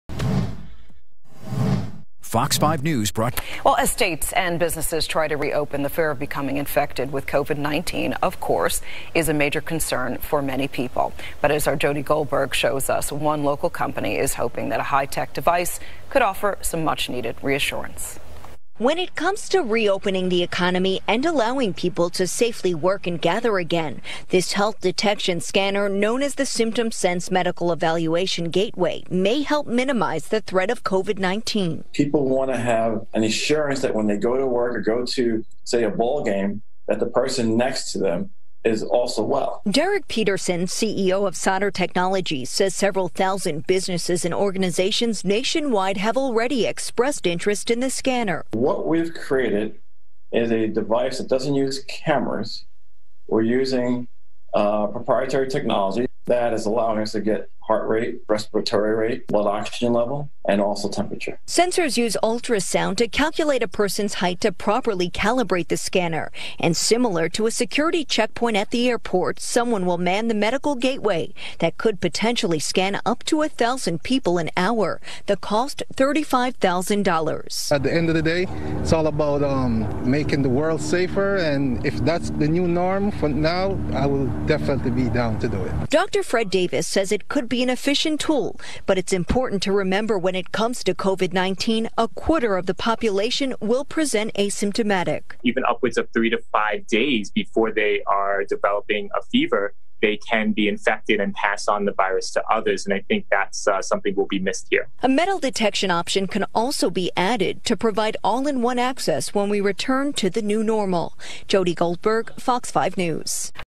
SymptomSense™ on Fox 5! The New Norm
Introducing the new norm. SymptomSense™ is a non-invasive, fast and easy vital screening tool to help minimize the spread of infections. SymptomSense™ is capable of screening for signs that correlate to known symptoms of illness: Blood Oxygen Level, External Body Temperature, Heart Rate, and Respiration Rate.